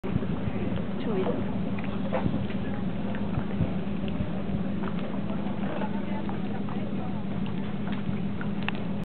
Canoa a pedali
Canoa a pedali, sarà anche normale ma non l'avevo mai vista...